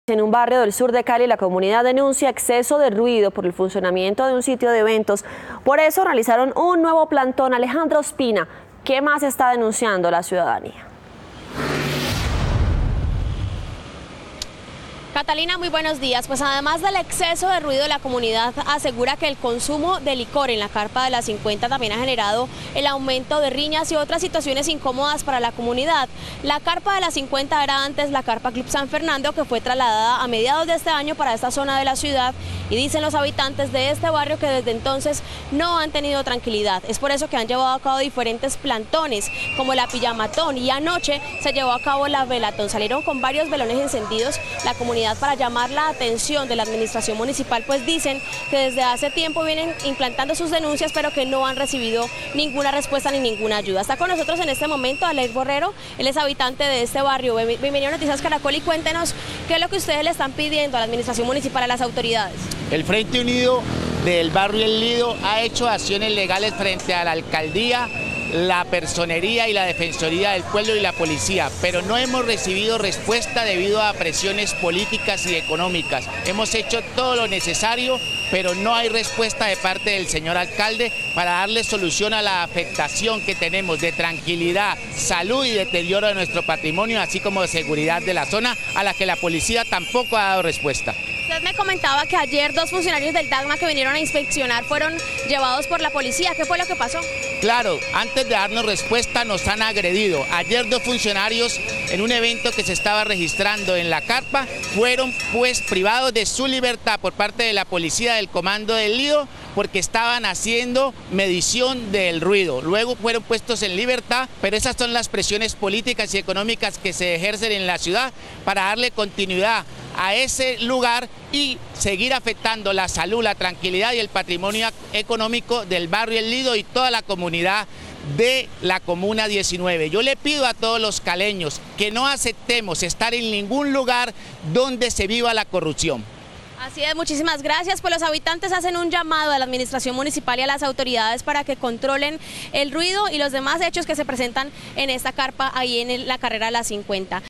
Con velones encendidos, ciudadanos del sur de Cali protestan por instalación de carpa de eventos
Según los habitantes, debido al consumo de licor en el lugar se ha presentado aumento en las riñas, además del exceso de ruido que, aseguran, les quitó la tranquilidad.